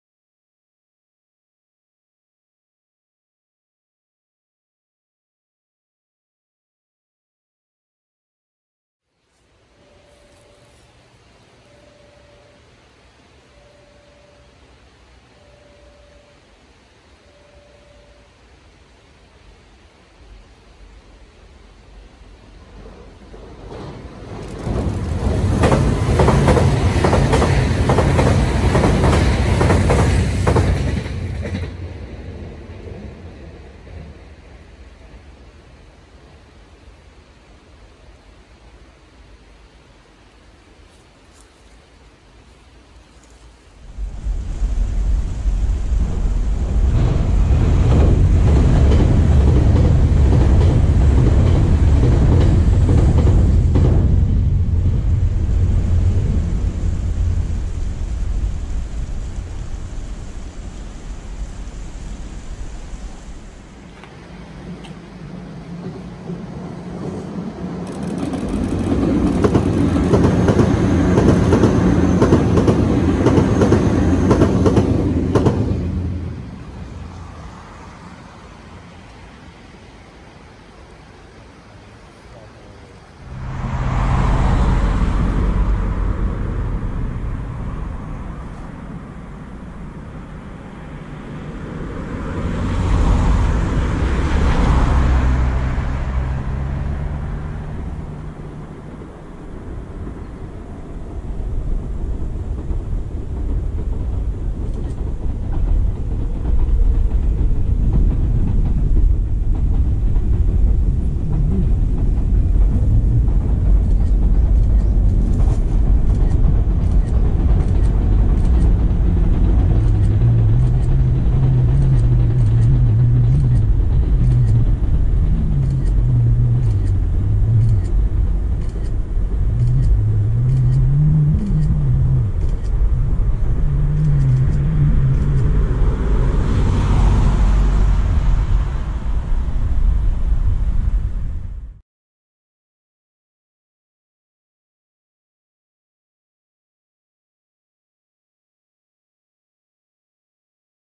【南海】 南海電車 臨時列車 特急 ラピート 高野線入線 ｜ Japanese Railway NANKAI line Express PASS
今日も臨時列車の特急ラピートの撮影して来ました。
今回は美加の台駅付近で上下それぞれ違う場所で♪
下から見上げる感じが好きです！！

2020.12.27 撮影